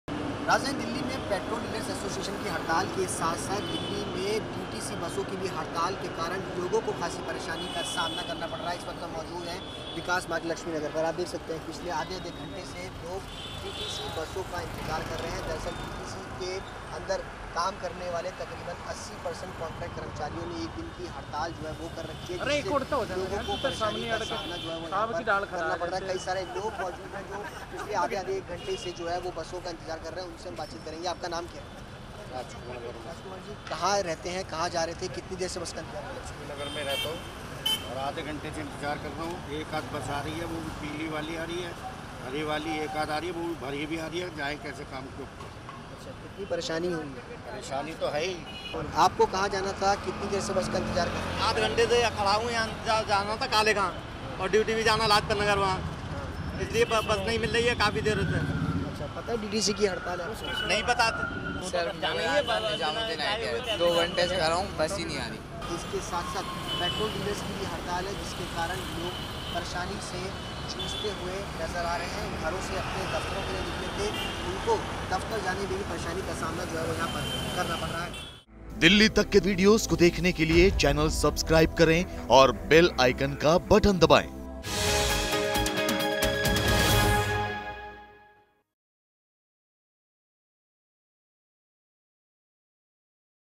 दिल्ली में डीटीसी के कॉन्ट्रैक्ट कर्मचारियों की हड़ताल| Dilli Tak
दिल्ली में डीटीसी के कॉन्ट्रैक्ट कर्मचारियों की हड़ताल के कारण लोगों को भारी परेशानी का सामना करना पड़ रहा है लोगों को ढाई घंटे तक डीटीसी बस का इंतजार करना पड़ा विकास मार्ग से डीटीसी बसों की हड़ताल पर जायजा लिया हमारे संवाददाता सुशांत मेहरा ने
About the channel: